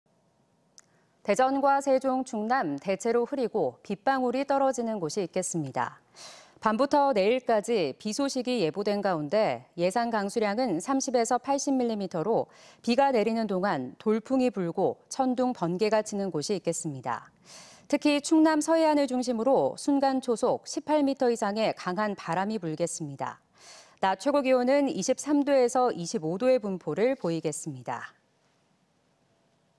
대체로 흐리고 곳에 따라 빗방울｜ TJB 대전·충남·세종뉴스
대전과 세종, 충남
대체로 흐리고
빗방울이
떨어지는 곳이 있겠습니다.

밤부터 내일까지 비 소식이 예보된 가운데
예상 강수량은 30~80mm로
비가 내리는 동안 돌풍이 불고
천둥 번개 치는 곳이 있겠습니다.

특히 충남 서해안을 중심으로
순간 초속 18m 이상의
강한 바람이 불겠습니다.

낮 최고기온은 23도에서 25도의 분포를
보이겠습니다.